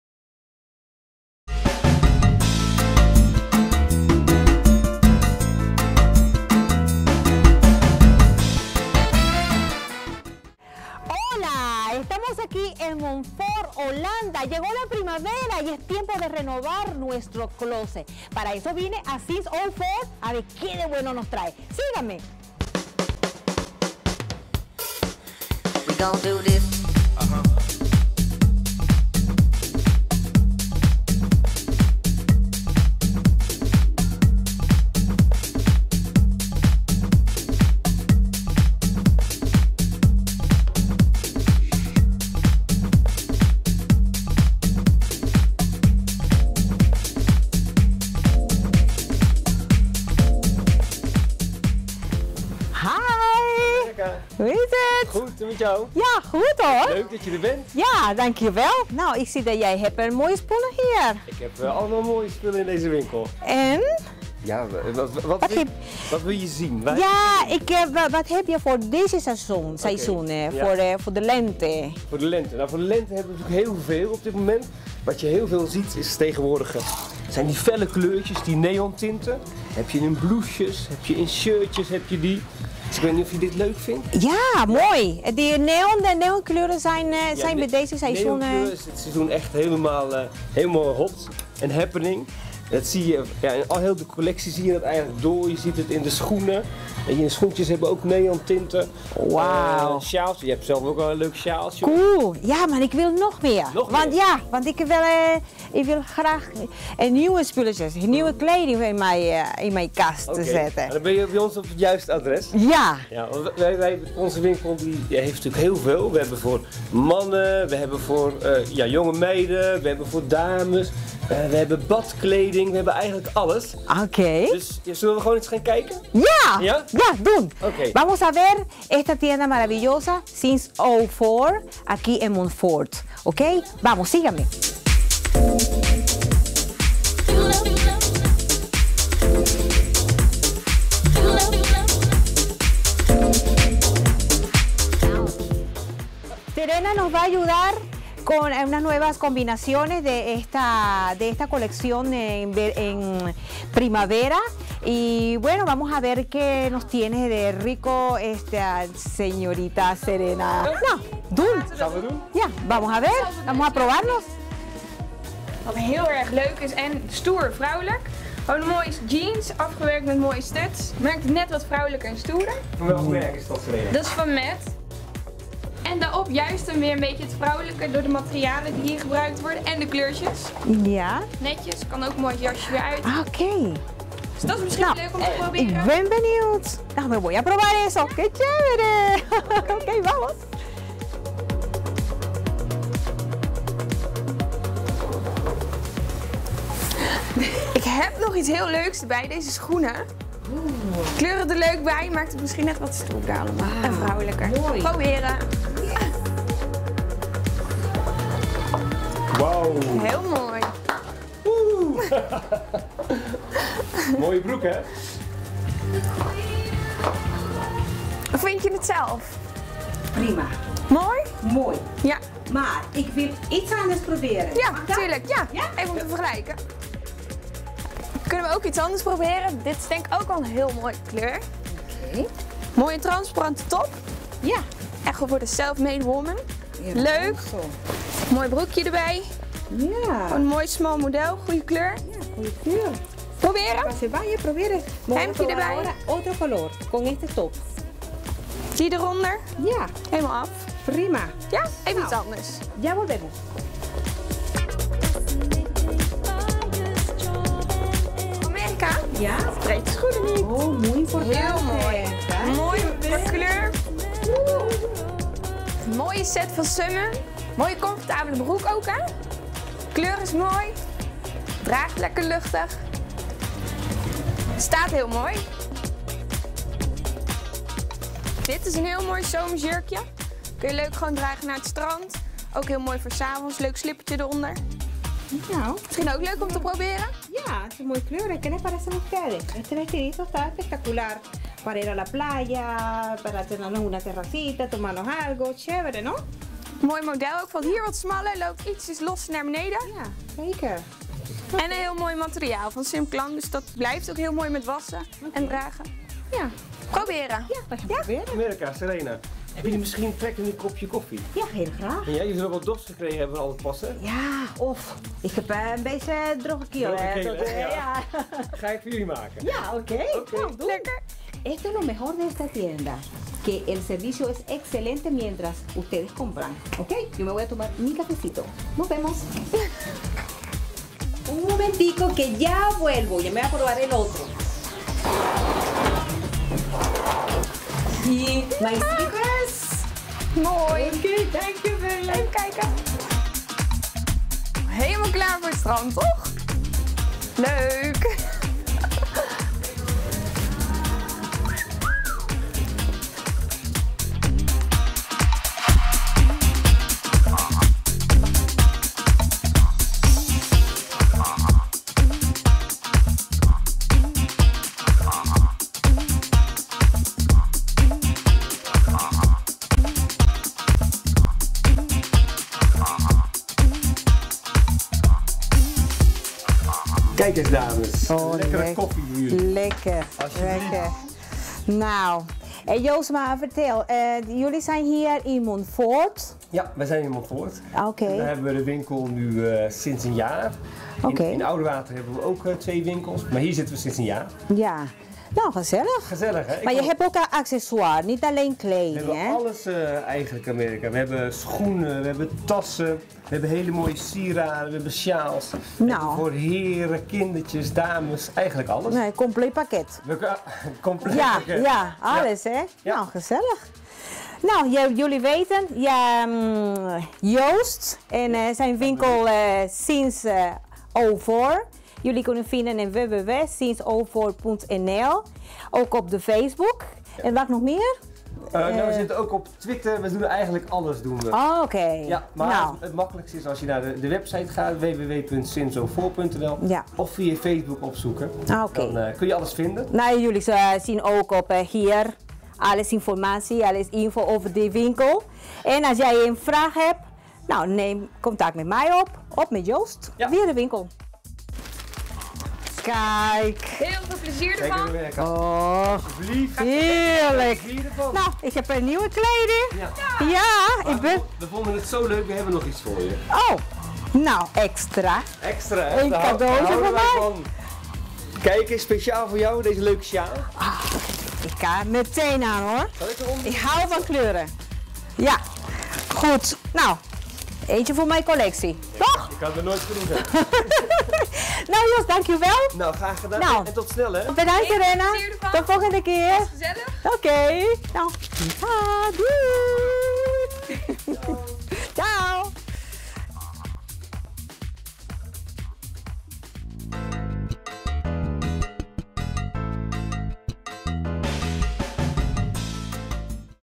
Spring Collection by Since04
Do you want to know what we have for this Spring Season? have a look in this video because Joos Middelman shows you what he has in his "Since 04" store, trendy cloths , fresh and beautifull.

Location: Since04 Store (Montfoort - The Netherlands)
Cameras: Monica Contreras
Production and Edition: Monica Contreras.